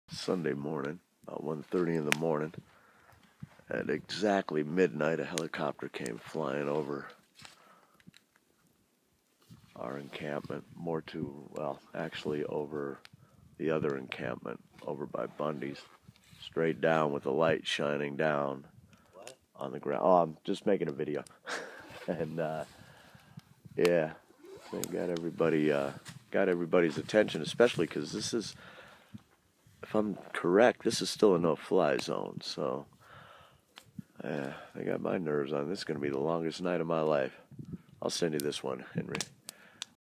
A brief report on the helicopter fly by in Bunkerville on Saturday
Wade gives us his report from Bunkerville on the helicopters flying over the area.